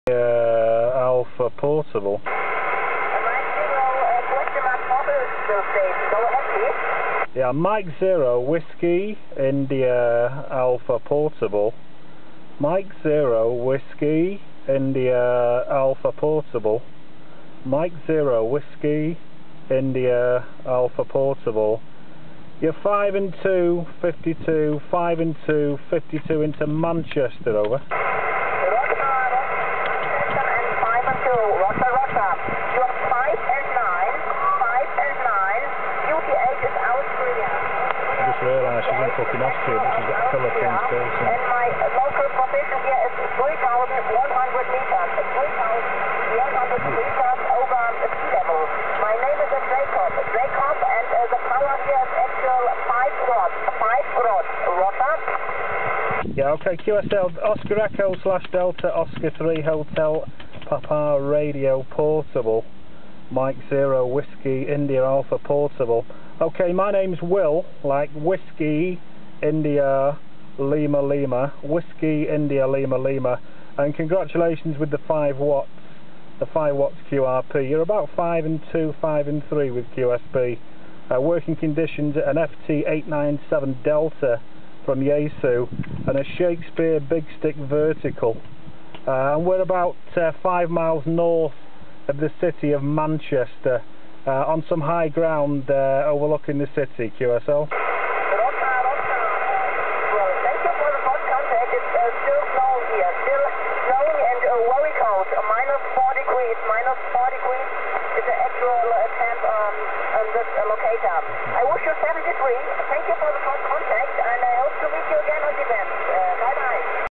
working a lady from the austrian alps on a mountain.
here is my amateur friend m0wia will  working a lady on 28.500 usb, she was  in the austrian alps who was qrp on 5 watts. she had the altitude to do it on qrp.

john